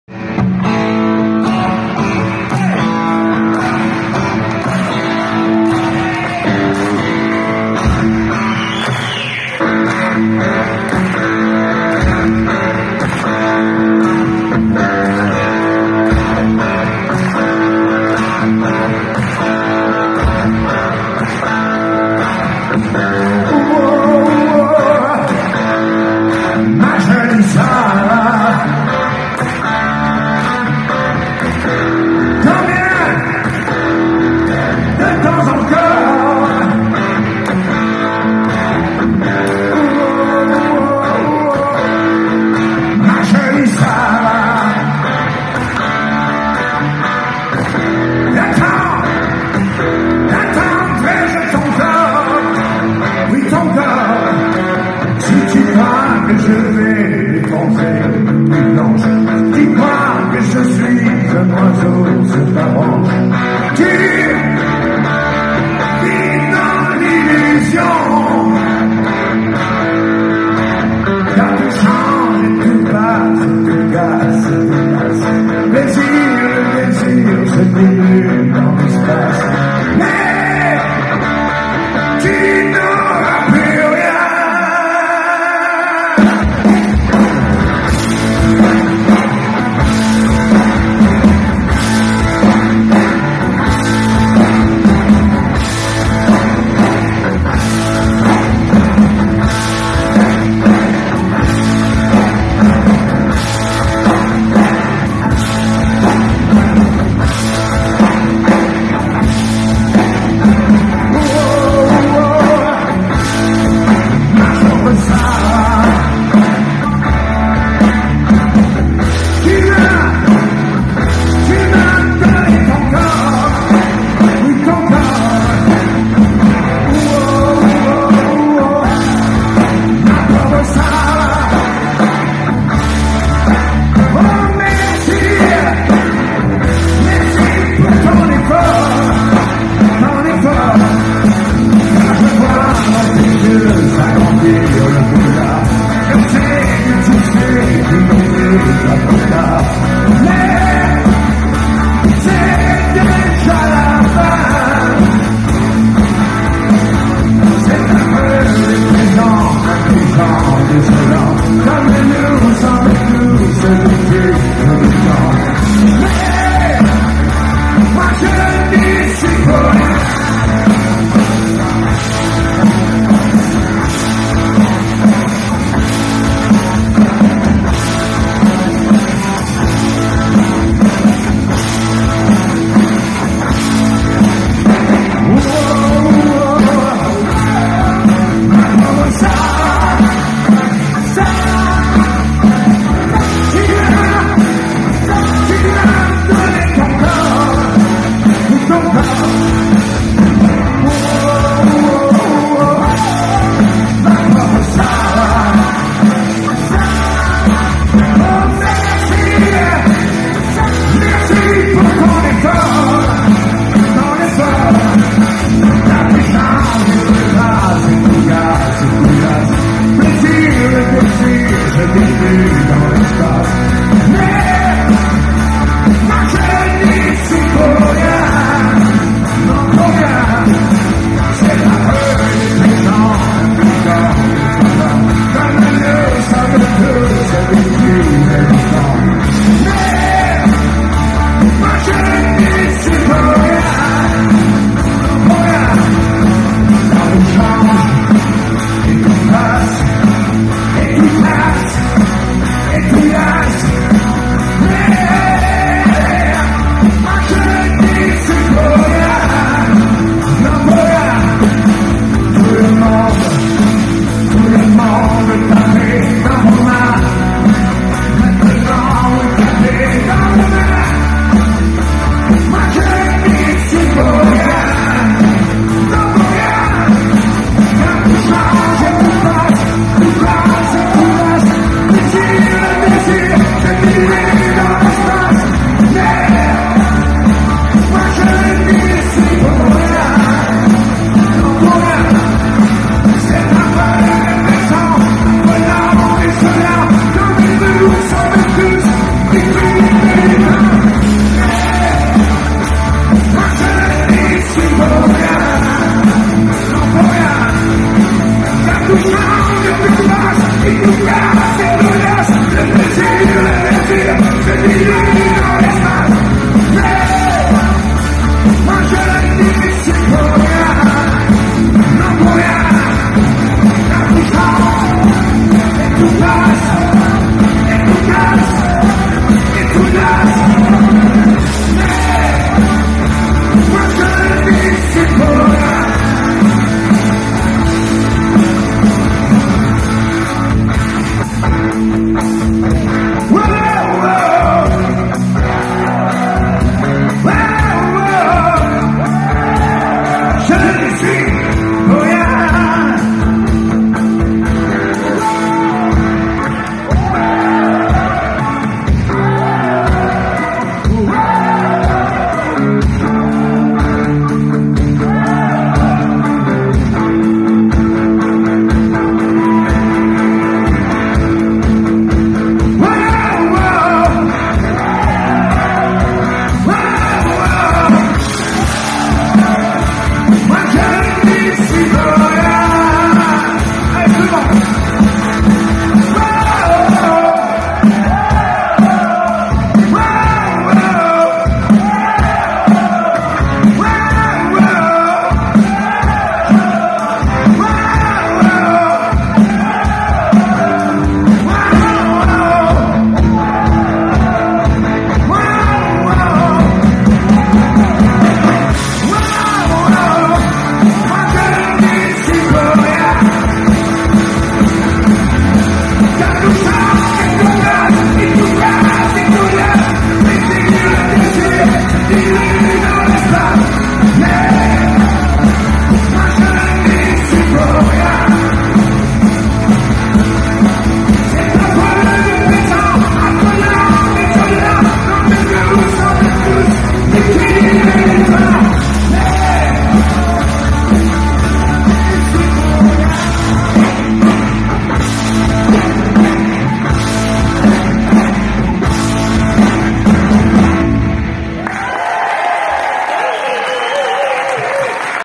joe arlandis.. concert avec ses musicos à montbartier
Sosie vocal de johnny hallyday